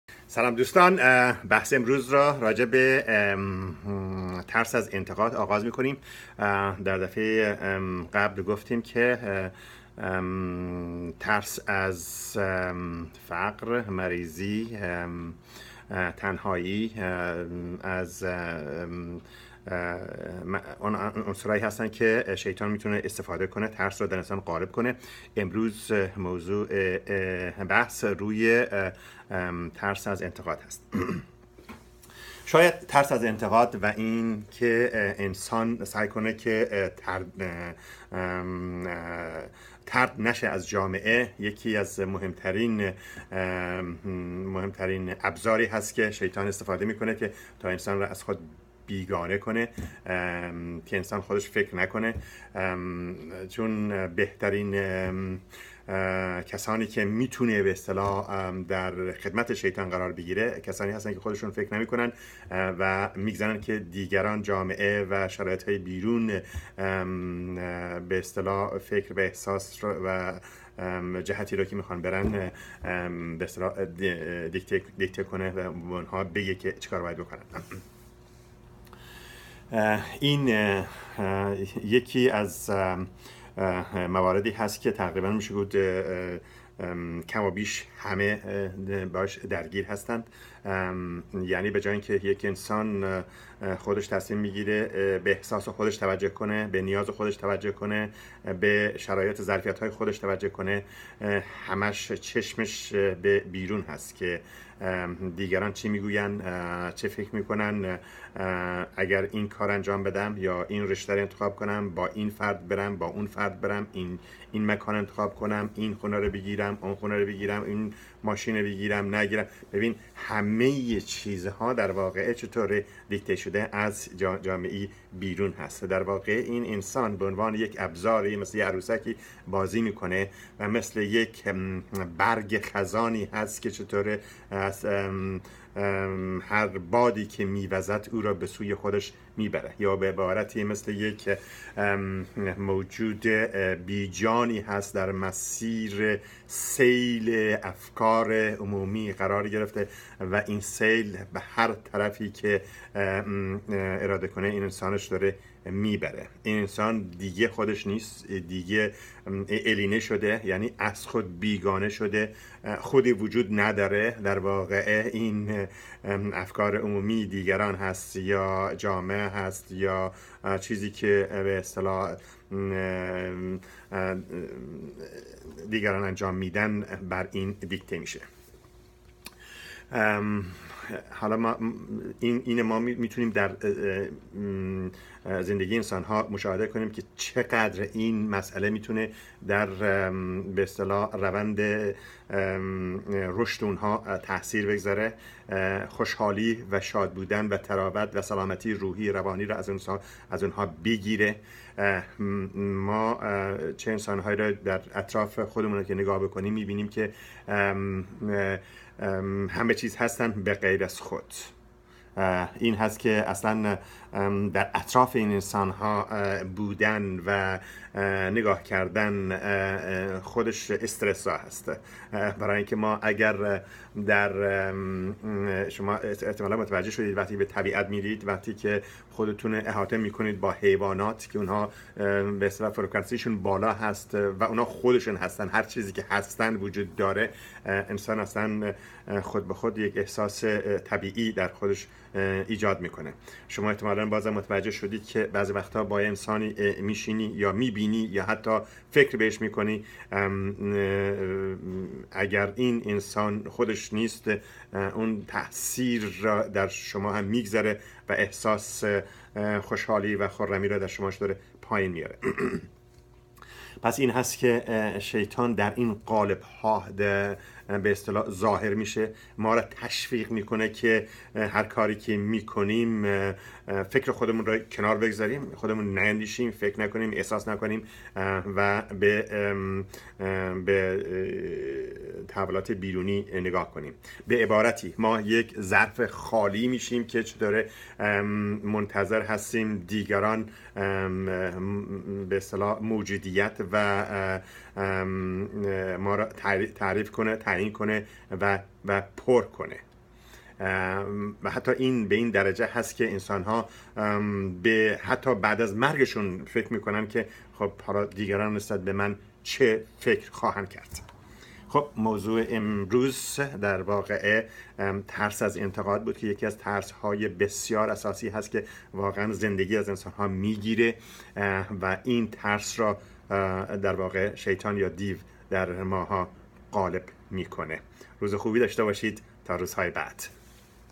پادشاهی شیطان یا دیو (ترس از ترد شدن در جامعه)
شیوه‌های حیله گونه رسوخ شیطان یا دیو در ما و چگونگی شناخت آن و بیرون راندن آن از خود